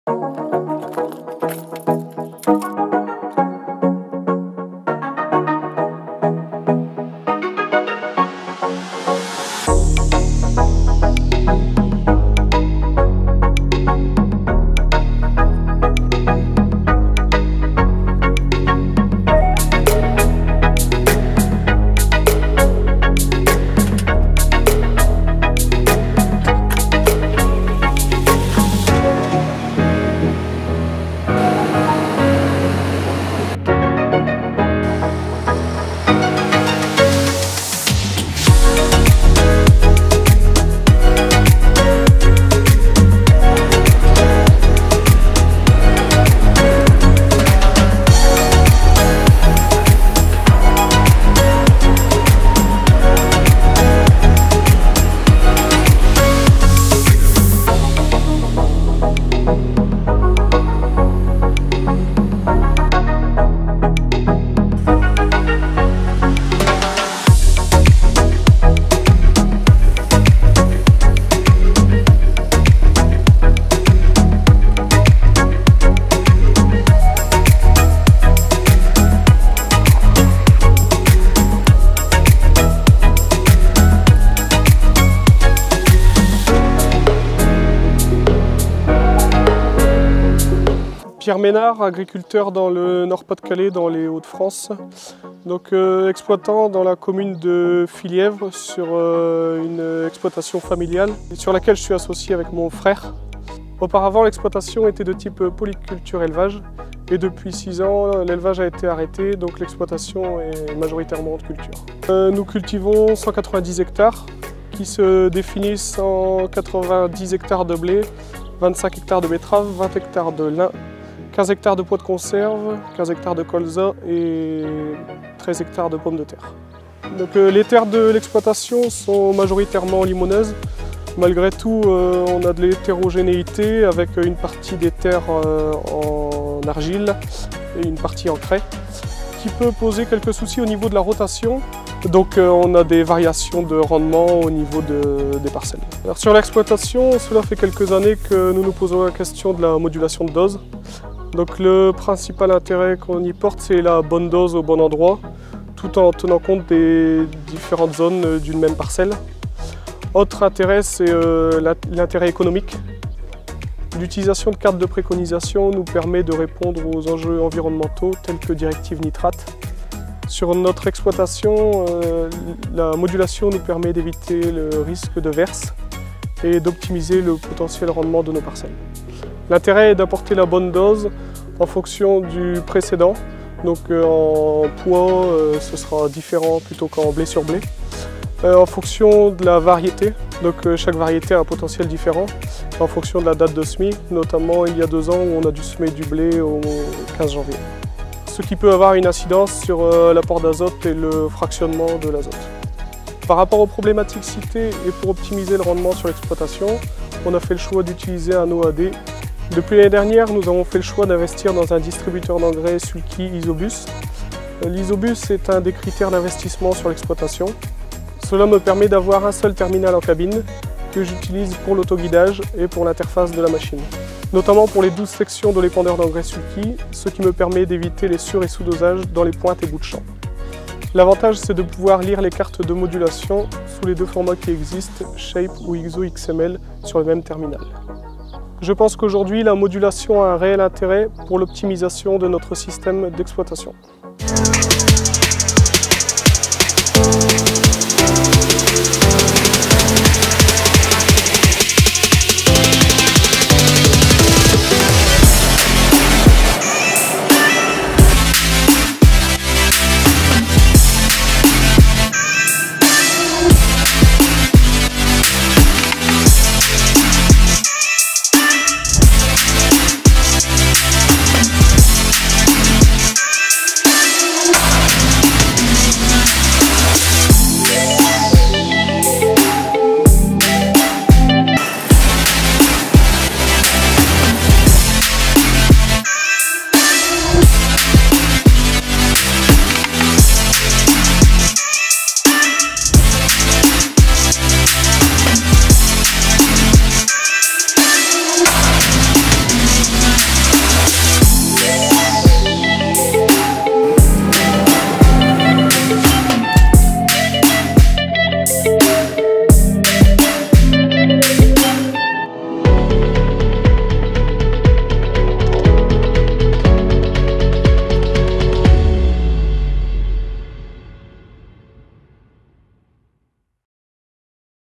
Moduler l'azote en 2019 ? Pierre nous explique pourquoi !
Sur l'exploitation de Pierre et Arnaud, Pierre nous explique pourquoi ils ont choisi de moduler leurs apports d'azote avec un OAD.

il souhaite apporter la bonne dose au bon endroit et notamment par rapport à la directive nitrate qui demande à limiter les apports.